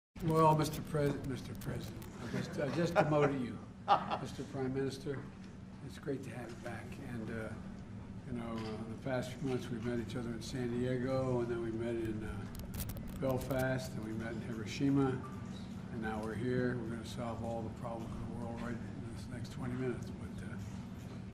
الرئيس الأمريكي جو بايدن يرتكب خطأ جديدا
الرئيس الأمريكي جو_بايدن يرتكب خطأ جديدا وينادي رئيس الوزراء البريطاني ريشي_سوناك بـ"الرئيس".